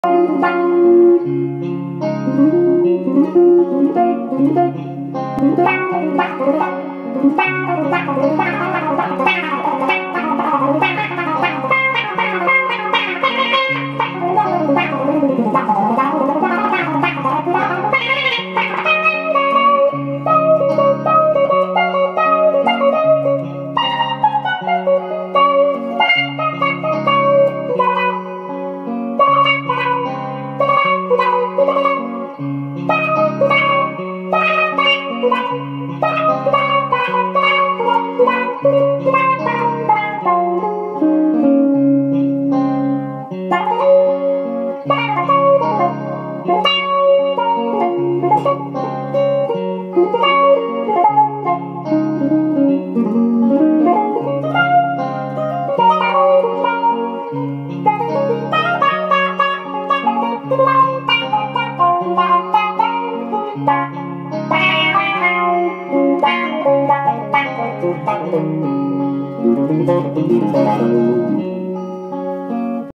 Steeldrum Tone
An interesting colour conceived with the Xotic Robotalk 2 into a Dr Z EZG 50 with David Gilmour Relic Strat.